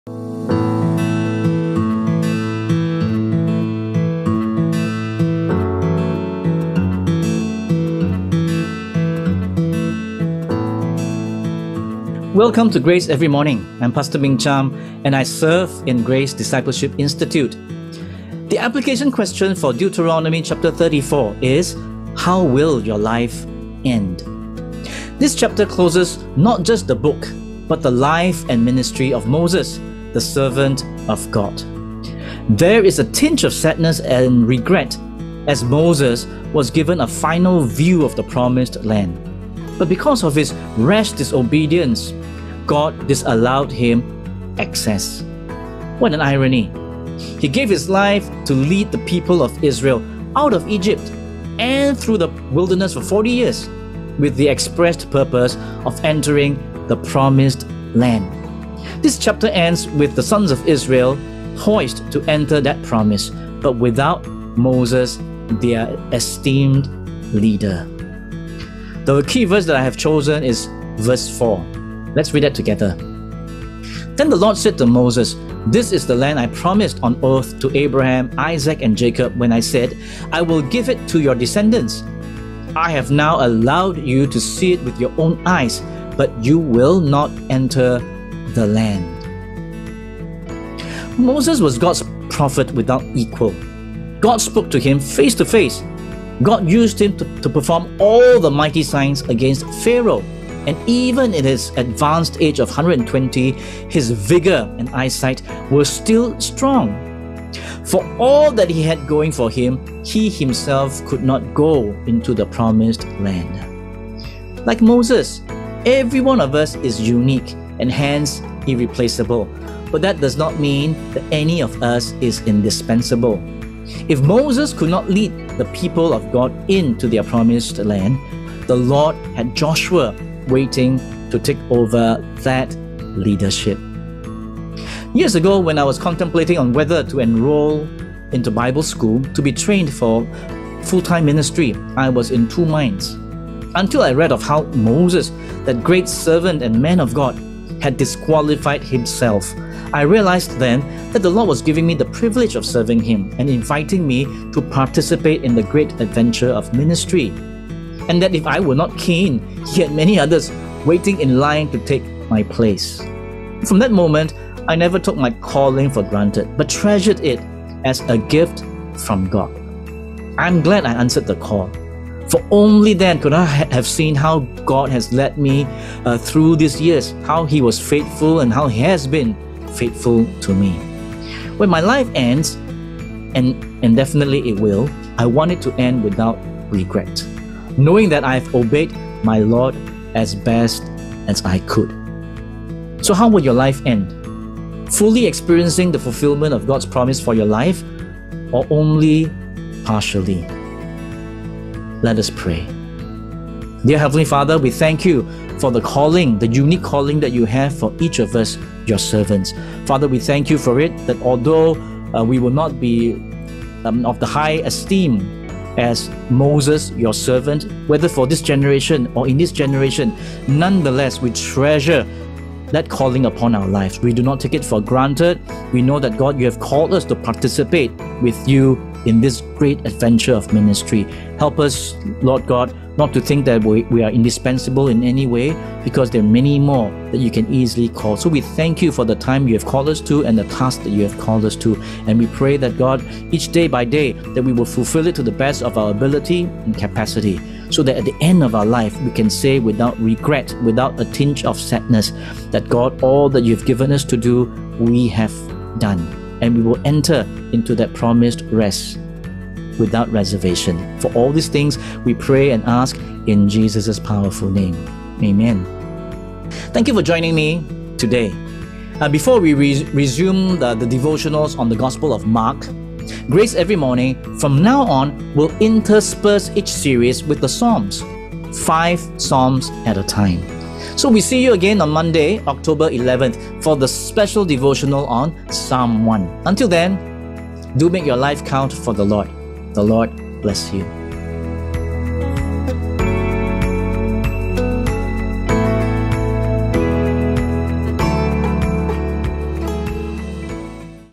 30 Sept 2021: Deuteronomy 34 - How will your life end?
30 Sept 2021
“How will your life end?” with Rev Meng Cham
Scripture reference: Deuteronomy 34

If you are new to Grace Assembly, we would like to know you better! Kindly help us to fill up our guest card